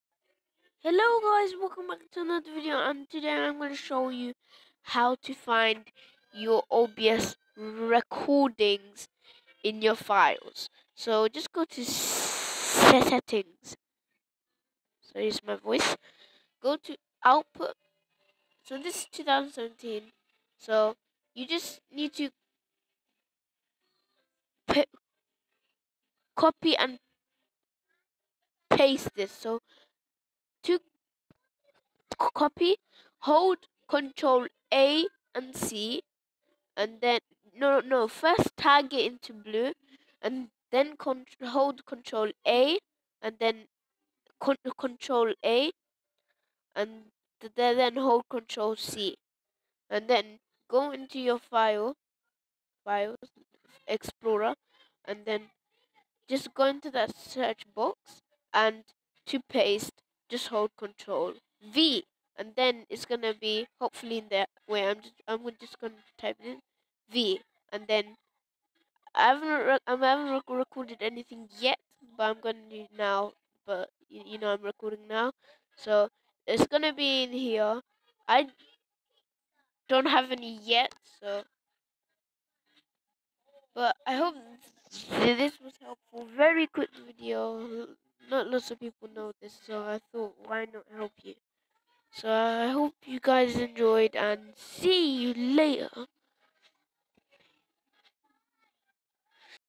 HOW TO FIND YOUR OBS RECORDINGS 2017!
Watch this cool video! : "Channel Trailer - 2019 Dadah Productions (Teaser)"

Hi Guys, today i'm going to show you how to find your obs recordings 2017!